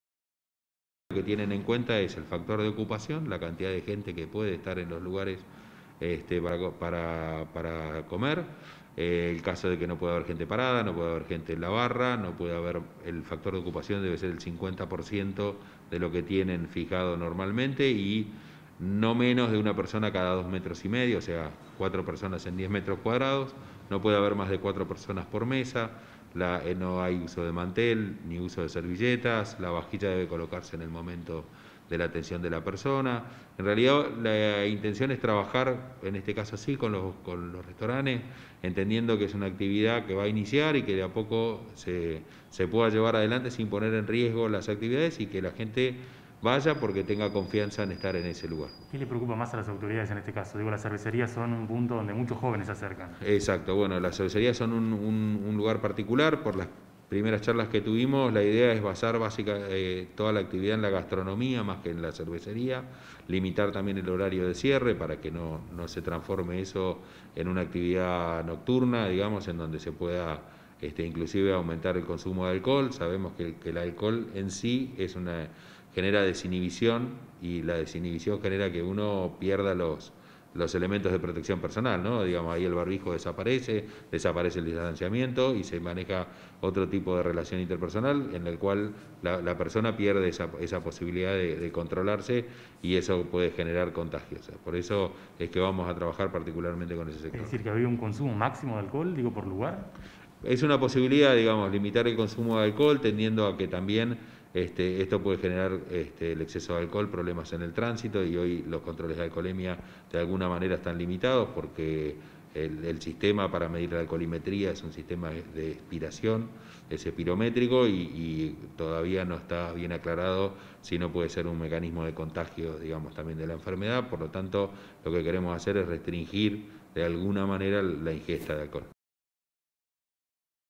Qué se evalúa para la reapertura de bares y restaurantes en Bahía
Desde el municipio de Bahía Blanca, se evalúa la reapertura de bares y reaturantes en el contexto de la nueva Fase 4.

Según afirmaron funcionarios, el protocolo que se prepara es estricto y consiste en disminuir la concurrencia de los bahienses, evitando las esperas y la aglomeración de personas. Asimismo, resaltar la exigencia del uso de tapabocas, la higiene, la desinfección y limpieza de manteles, mesas, etc.

A raíz de las propuestas establecidas para la apertura de cervecerías y bares, surge nuevamente la inquietud respecto a los controles de alcoholemia, tema que sigue en materia de discusión.

Jouglard adelantó que una de las medidas que evalúan sería colocar un límite en el consumo de alcohol para evitar los test de alcoholemia y la propagación del virus